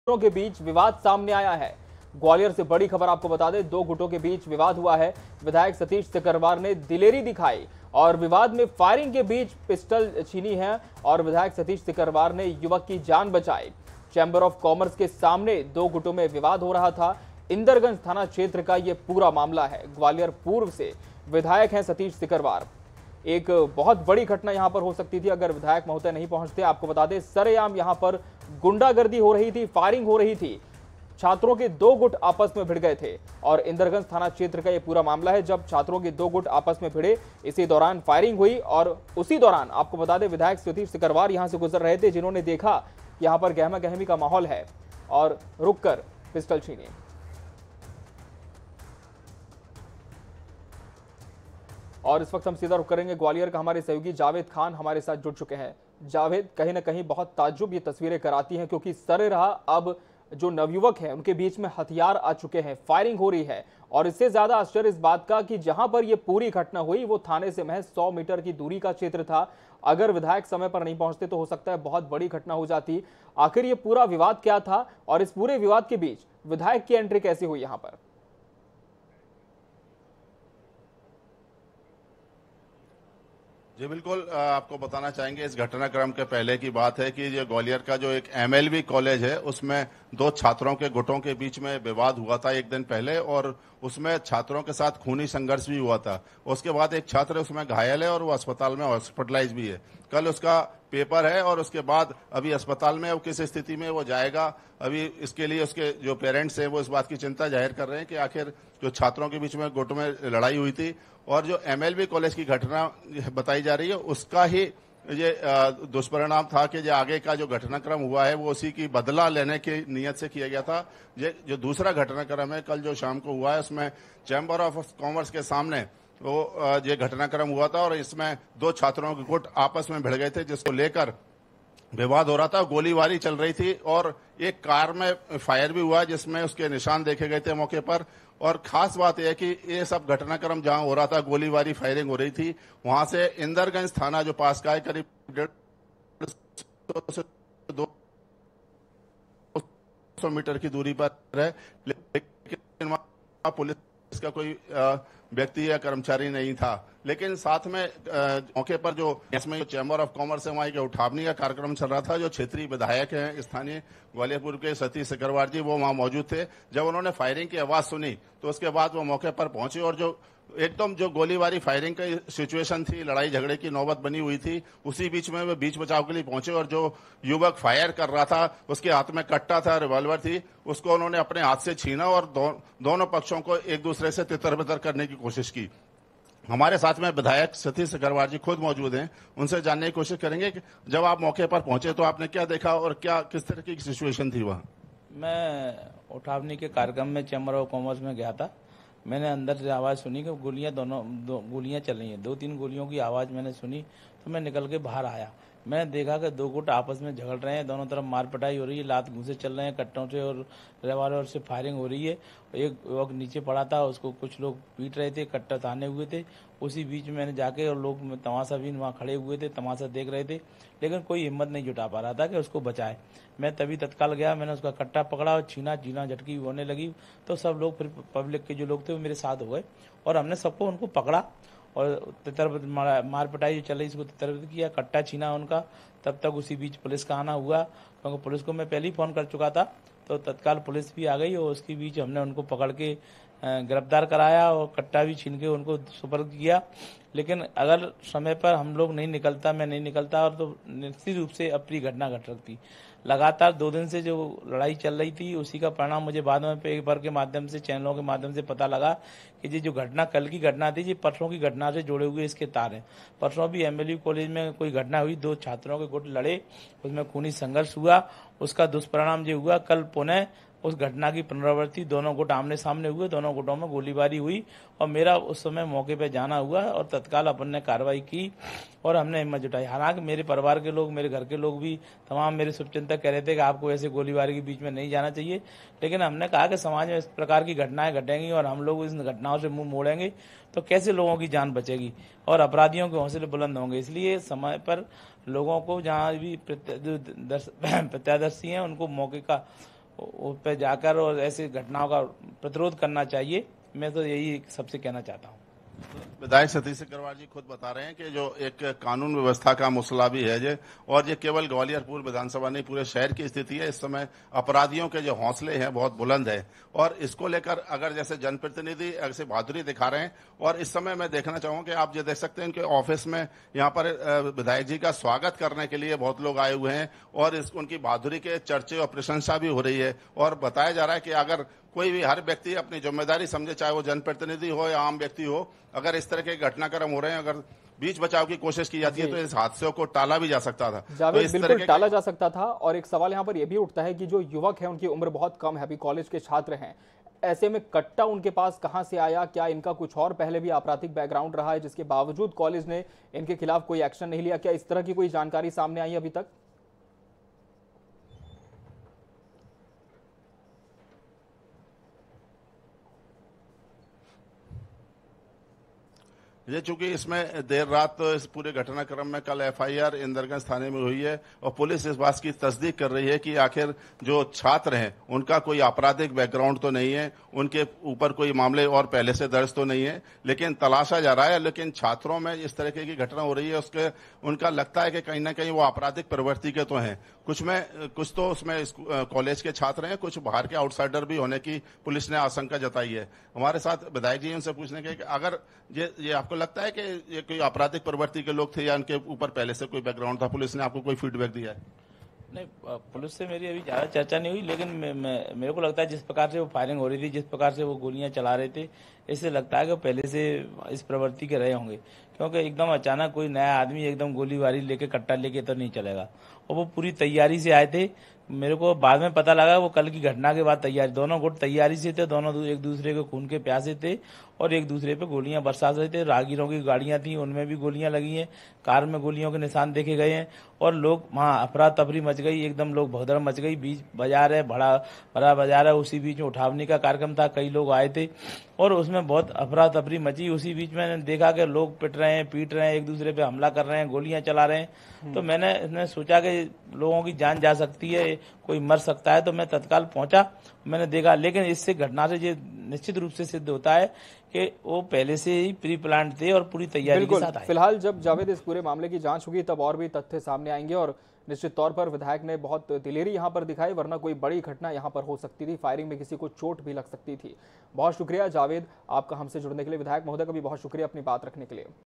MP News || 2 गुटों के बीच विवाद MLA Satish Sikarwar ने दिखाई दिलेरी, INH 24X7 से की खास बातचीत
2 गुटों के बीच विवाद MLA Satish Sikarwar ने दिखाई दिलेरी, INH 24X7 से की खास बातचीत

Madhya Pradesh Gwalior News,
Madhya Pradesh Latest News,
Gwalior News,
Gwalior Latest News,
CMO Madhya Pradesh,
Madhya Pradesh Government,
Latest News,